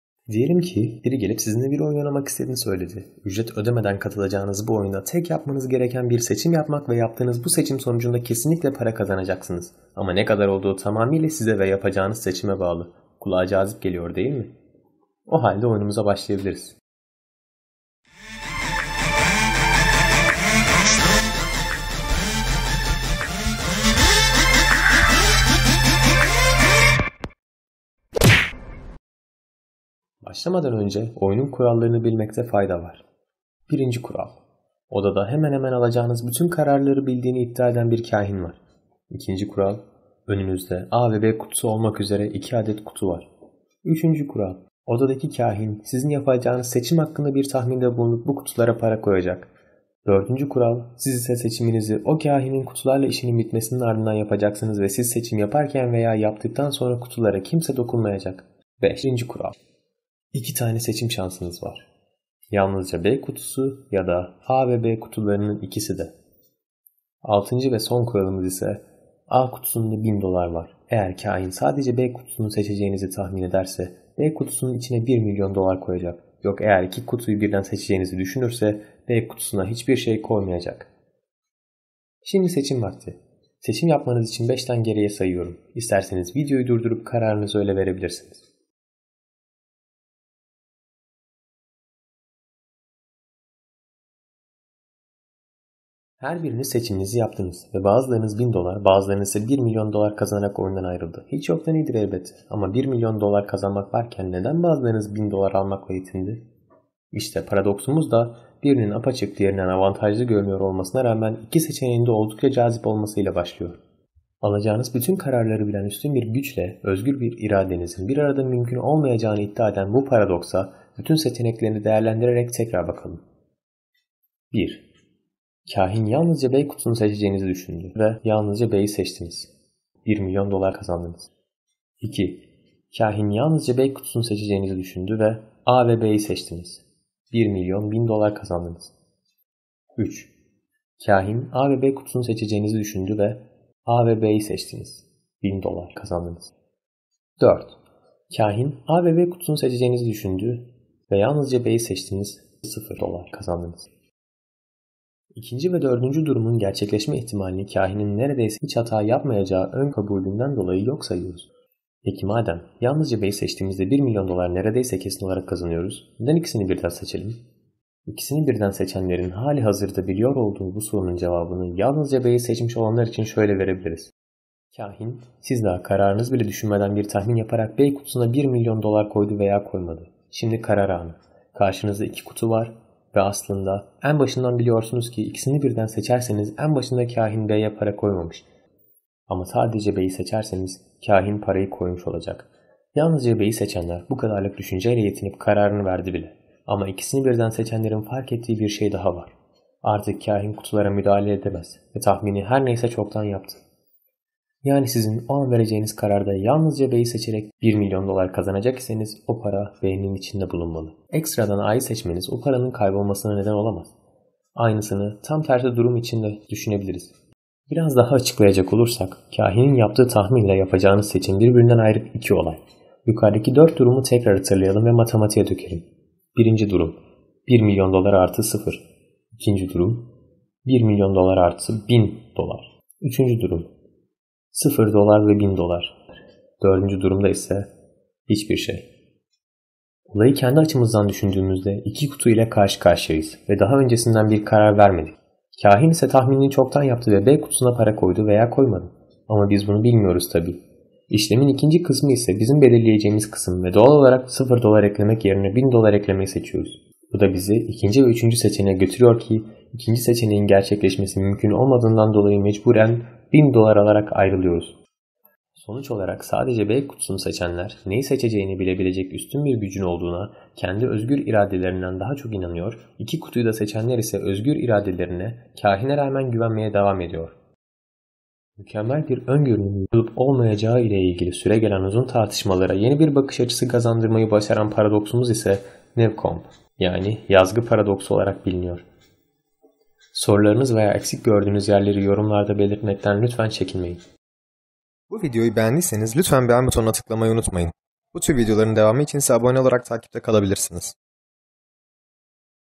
Seçimlerimizde Ne Kadar Söz Sahibiyiz? - Newcomb Paradoksu
- Alacağımız bütün kararları bilen üstün bir güç ile özgür irademizin birarada olması mümkün müdür?

- 1,000,000 dolar kazanmak varken neden 1,000 dolar kazanmayı tercih ederiz?

- Seçimlerimizde tam kontrol sahibi miyiz?

  Mükemmel bir öngörünün olup olmayacağı ile ilgili süregelen uzun tartışmalara yeni bir bakış açısı kazandırmayı başaran Newcomb(Yazgı) Paradoksu, biz insanları iki tane cazip seçenek arasında bırakıyor. İlk bakışta bir tanesi apaçık mantıklı gözüküyor olsa bile aslında ikisinin de neden cazip olduğu videomuzda detaylı bir şekilde anlatılıyor.

  Anlaşılması zor bir konu olduğundan dolayı anlamadığınız yerler olabilir, aklınızı kurcalayan her türlü soruyu yorumlarda sorabilirsiniz 😊

Bu paradox ile ilgili daha fazla bilgi için; Amazon Kindle (e-book) üzerinden "Destiny: It Is A Concept That Fills Men Doubt" kitabına linkinden ulaşabilir ve dilerseniz kitabı ücretsiz okuyabilirsiniz.